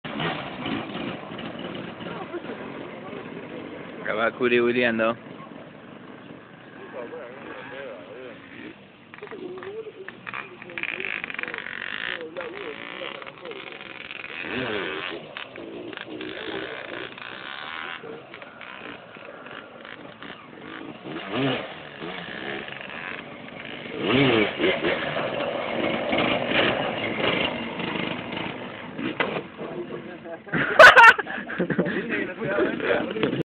curi colgando cuatriciclo
curi gaby y adri en un dia de recreacion cuatriciclica... jajaja